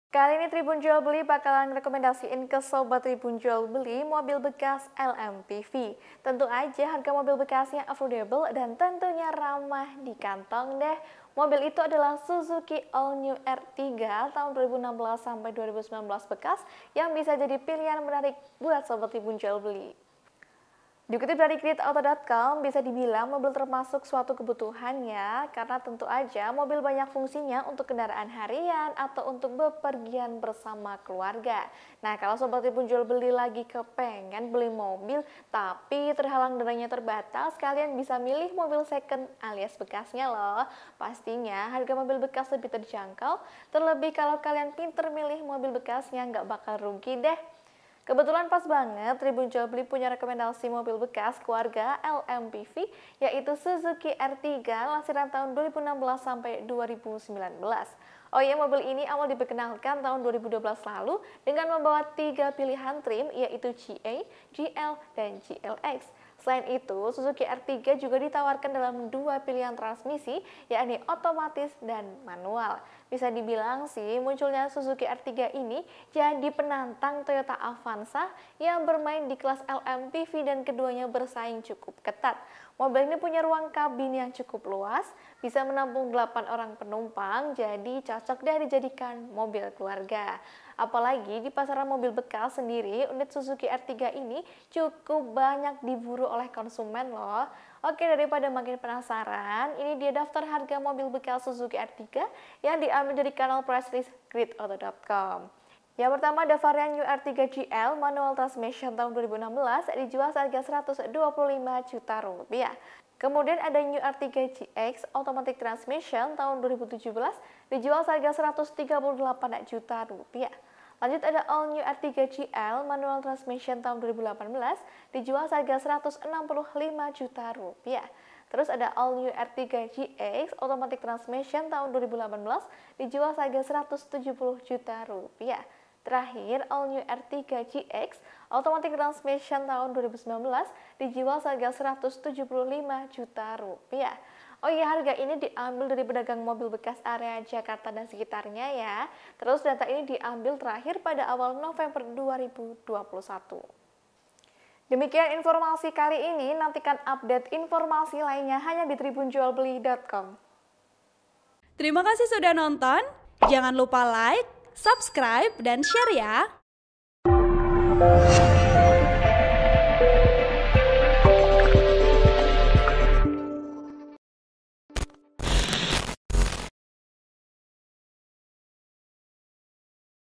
REHAT: Update Harga Suzuki Ertiga 2016-2019 Bekas, Harganya Makin Murah Mulai Rp 100 Jutaan
TRIBUN-VIDEO.COM - Kali ini Tribunjualbeli bakalan ngrekomendasiin ke sobat Tribunjualbeli mobil bekas LMPV.

Tentu aja harga mobil bekasnya affrodable dan tentunya ramah di kantong.

Mobil itu adalah Suzuki All New Ertiga tahun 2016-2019 bekas yang bisa jadi pilihan menarik buat sobat Tribunjualbeli.

Dikutip dari Gridoto.com, bisa dibilang mobil termasuk suatu kebutuhan ya.

Karena tentu aja mobil banyak fungsinya untuk kendaraan harian atau untuk bepergian bersama keluarga.

Kalau sobat Tribunjualbeli lagi kepengen beli mobil tapi terhalang dananya terbatas, kalian bisa milih mobil seken alias bekas.

Pastinya harga mobil bekas lebih terjangkau, terlebih kalau kalian pinter milih mobil bekasnya nggak bakalan rugi.

Kebetulan pas banget Tribunjualbeli punya rekomendasi mobil bekas keluarga LMPV yaitu Suzuki Ertiga lansiran tahun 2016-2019.

Ohiya, mobil ini awal diperkenalkan tahun 2012 lalu dengan membawa 3 pilihan trim yaitu GA, GL dan GLX.

Selain itu Suzuki Ertiga juga diwarkan dalam 2 pilihan transmisi yakni otomatis dan manual.

Bisa dibilang munculnya Suzuki Ertiga ini jadi penantang Toyota Avanza yang bermain di kelas LMPV dan keduanya bersaing cukup ketat.

Mobil ini punya ruang kabin yang cukup luas, bisa menampung 8 orang penumpang jadi cocok dijadikan mobil keluarga.

Apalagi di pasar mobil bekas (mobkas) sendiri, unit Suzuki Ertiga cukup banyak diburu oleh konsumen.

Daripada makin penasaran, ini dia daftar harga mobil bekas Suzuki Ertiga yang diambil dari kanal pricelist GridOto.com.

Pertama ada varian New Ertiga GL M/T 2016 Rp 125 juta.

Kemudian ada New Ertiga GX A/T 2017 Rp 138 juta.

Lanjut All New Ertiga GL M/T 2018 Rp 165 juta.

Terus ada All New Ertiga GX A/T 2018 Rp 170 juta.

Terakhir All New Ertiga GX A/T 2019 Rp 175 juta.

Ohiya, harga ini diambil dari pedagang mobil bekas area Jakarta dan sekitarnya ya.

Terus data ini diambil terakhir pada awal November 2021.

(Tribun-Video.com)

Artikel ini telah tayang di Gridoto.com, dengan judul "Update Harga Mobil Bekas Suzuki Ertiga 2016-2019, Eh Sekarang Mulai Rp 100 Jutaan Nih"